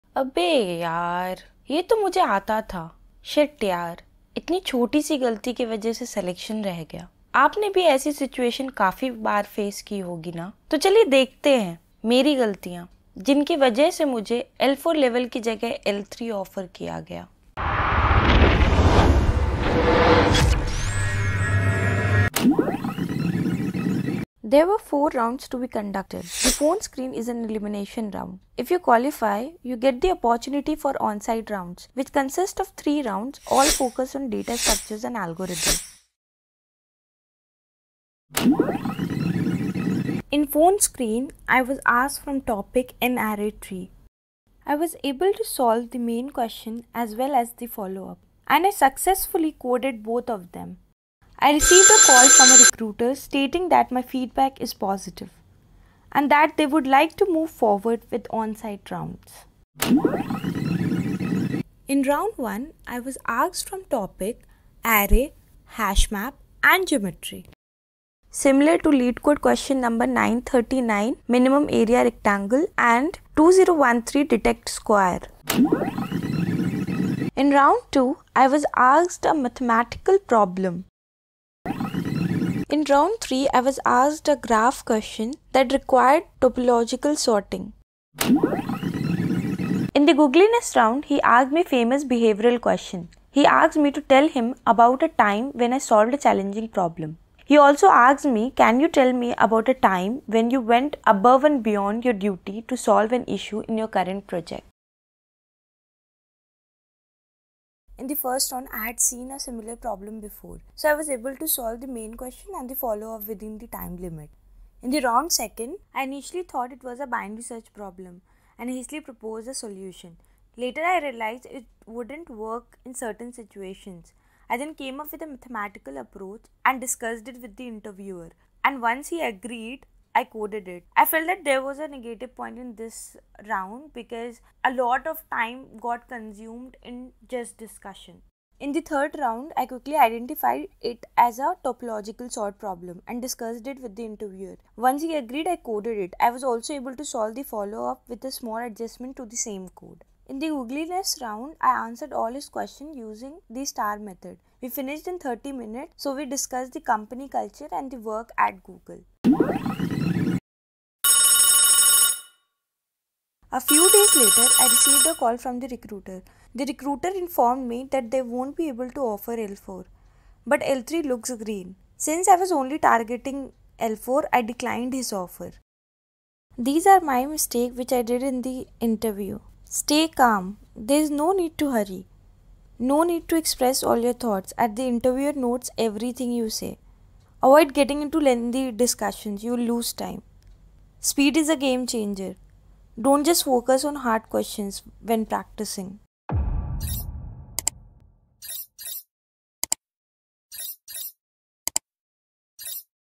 Google Interview Experience 2024 | SDE Google Interview Process
In this video , I am sharing my Google interview experience , I have covered google interview process , google phone screen and google onsite rounds in detail.
I have also shared my mistakes which I made during google preparation and during interview .
I have shared google interview questions topic as well as some leetcode similar problems.

Timeline
00:00 Introduction
00:23  Interview Process
00:38 Phone Screen
1:00   DSA Round 1
1:16   DSA Round 2
1:22   DSA Round 3
1:29  Googlyness Round
1:45  My Review